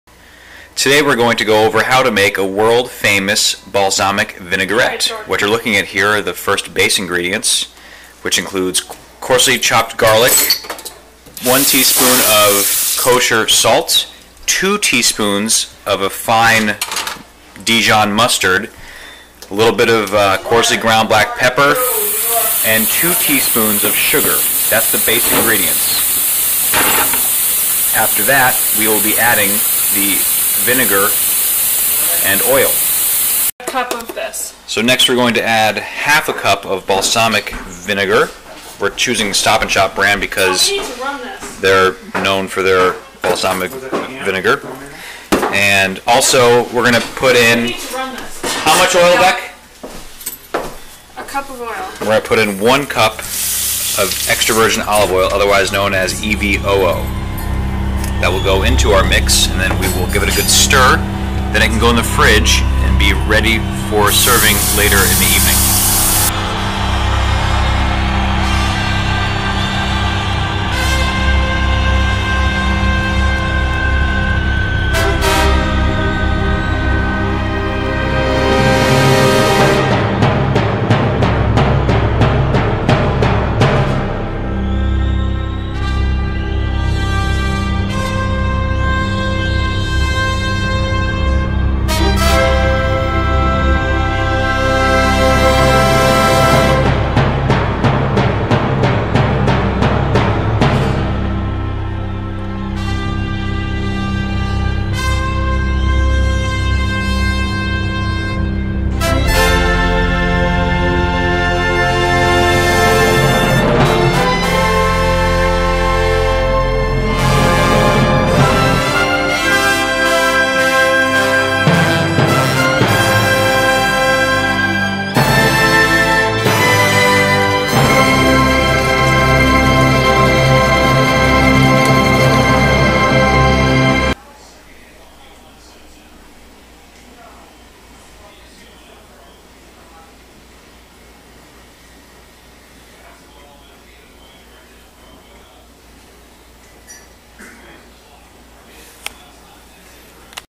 Balsamic Vinaigrette - Worlds Best - by Becky (Dolcetti) Lewis
actually this is based on Emril's recipe.   It's very good.  Enjoy.  Watch in HD if you have a fast computer/connection.